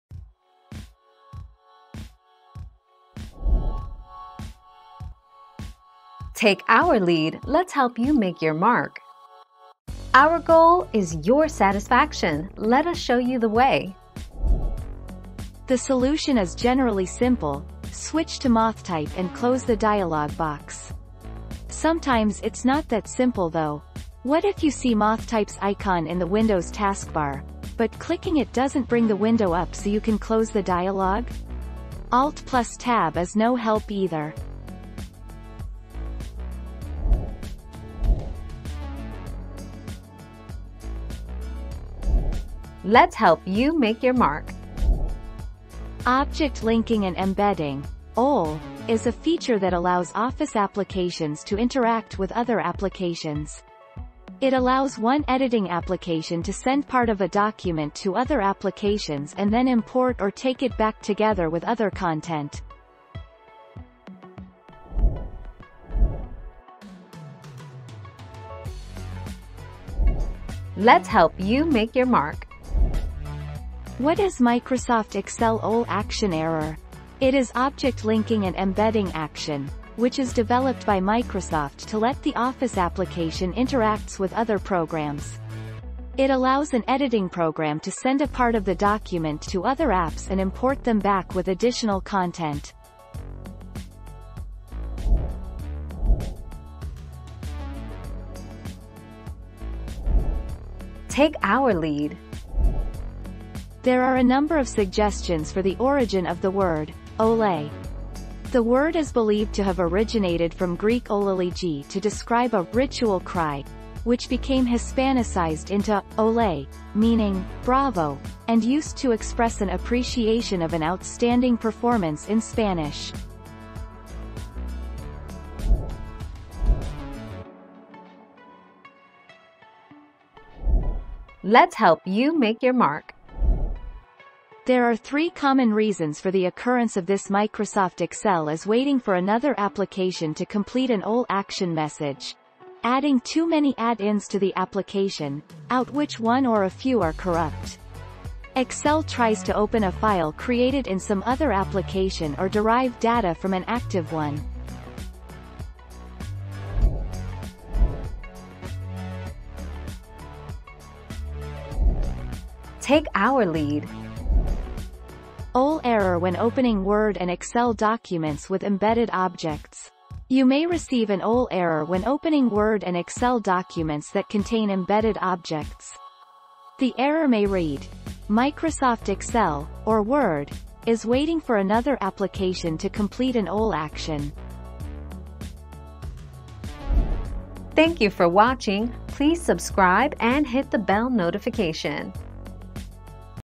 How do you solve the words waiting for an OLE action to finish?
00:00 - How do you solve the words waiting for an OLE action to finish?
00:38 - What is an OLE action in Microsoft?
01:06 - What is OLE action error?
01:36 - What does Ole mean?
02:07 - Is waiting for another application to complete an OLE action?
02:38 - What is an Ole error?

Laura S. Harris (2021, June 21.) How do you solve the words waiting for an OLE action to finish?
    AskAbout.video/articles/How-do-you-solve-the-words-waiting-for-an-OLE-action-to-finish-255244